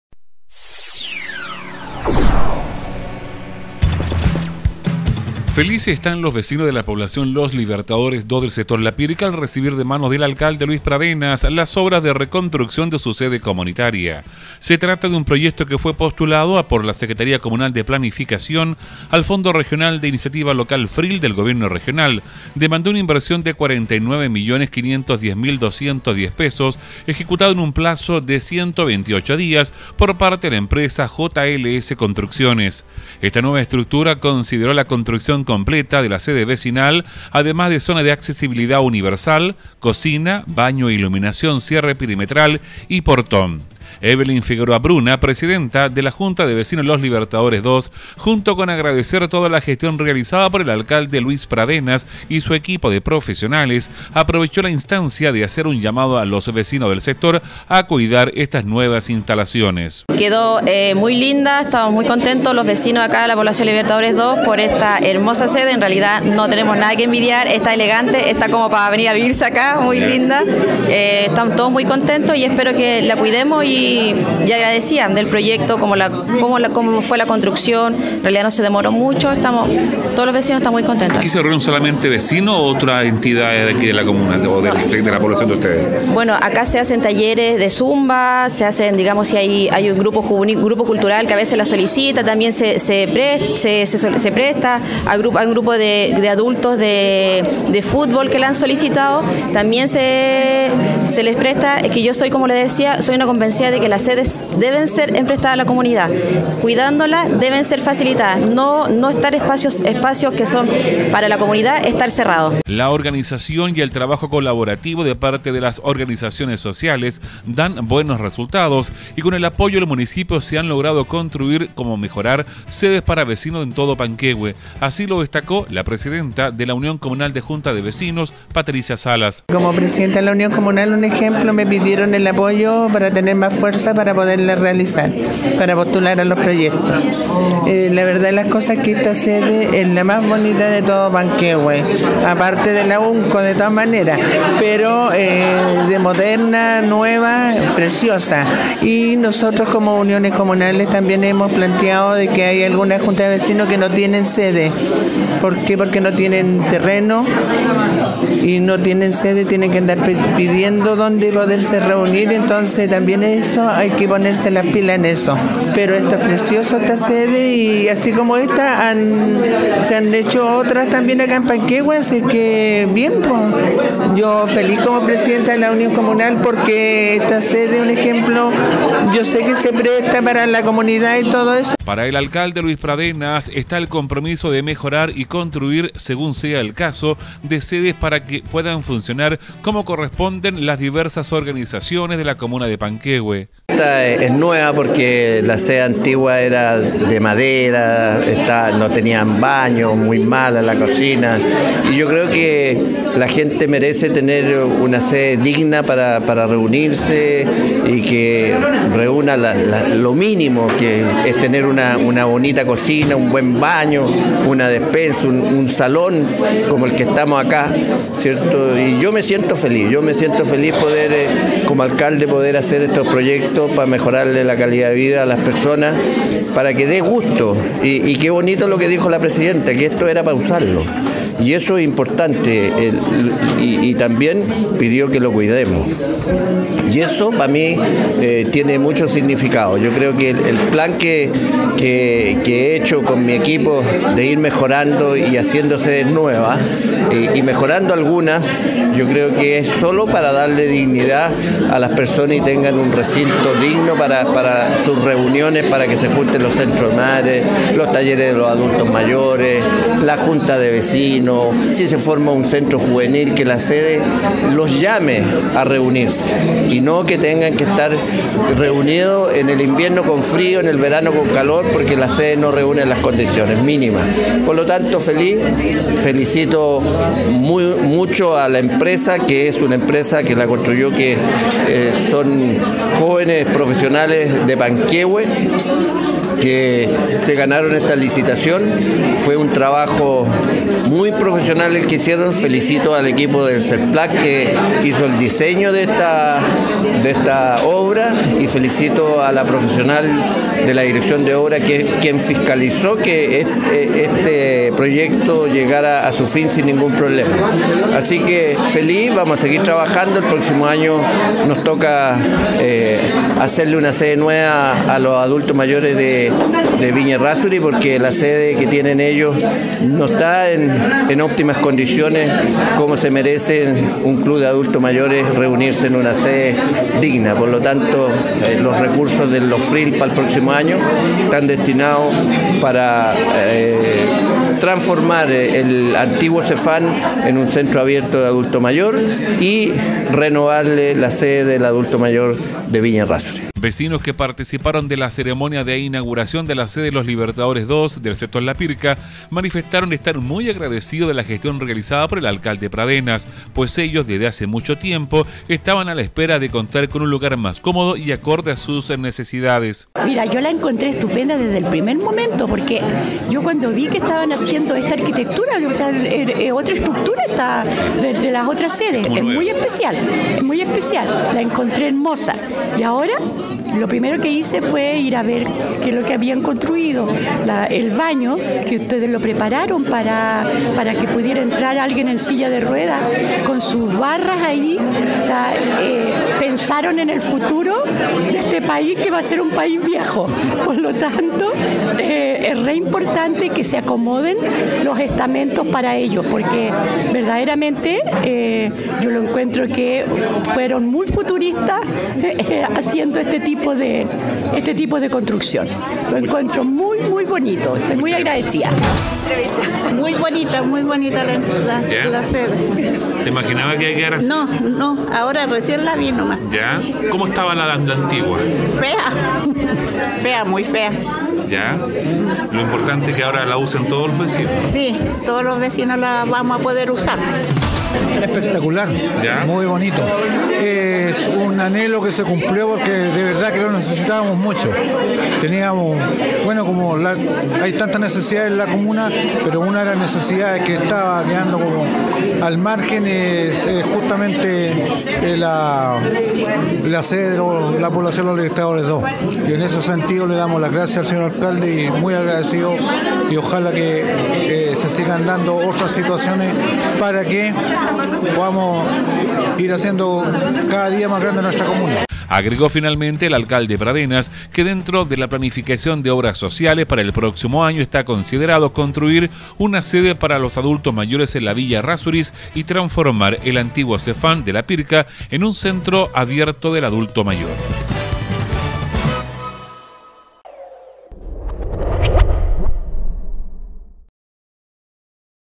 Inauguracion Sede Los Libertadores II
Felices están los vecinos de la Población Los Libertadores II del sector La Pirca, al recibir de manos del alcalde Luis Pradenas, las obras de reconstrucción de su sede comunitaria.